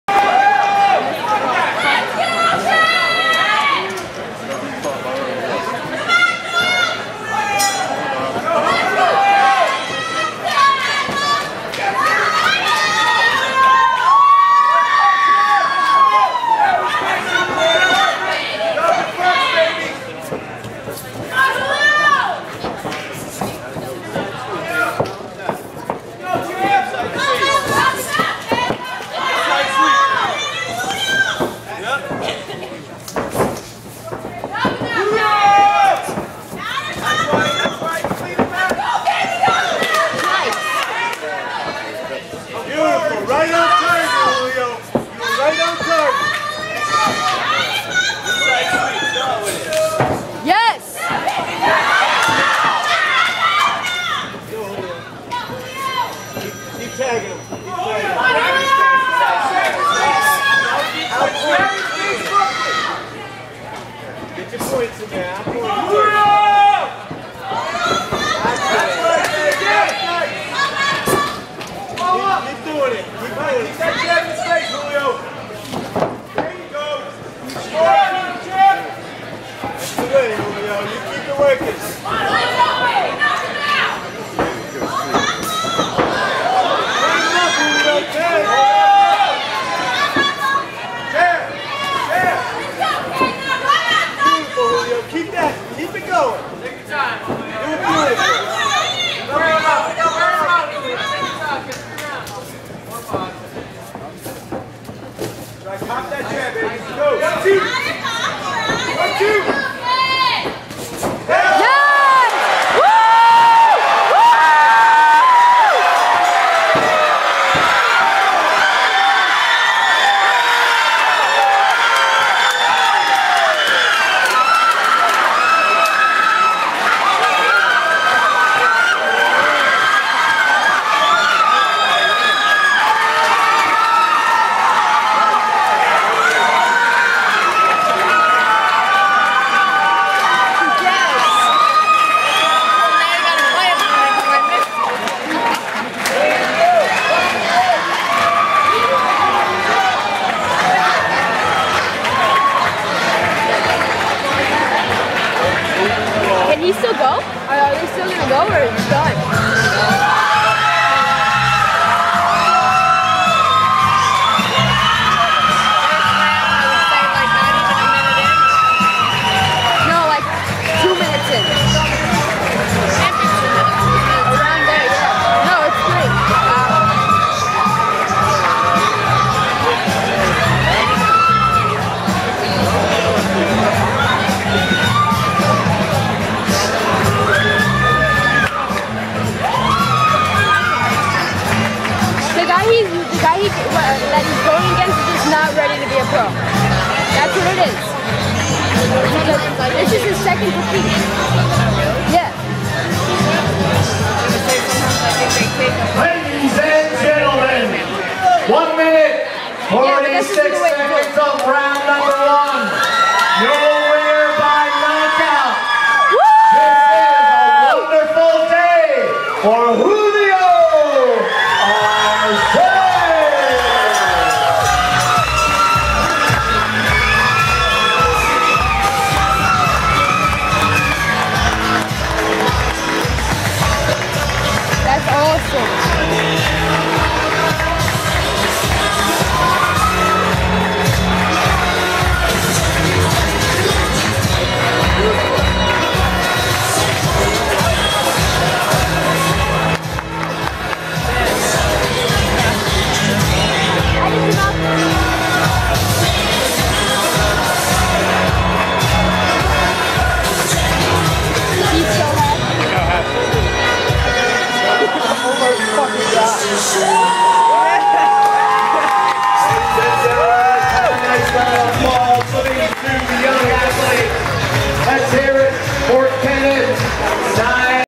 Julio arce first pro debut
July 13 2012 in philly great knock out  for his first pro fight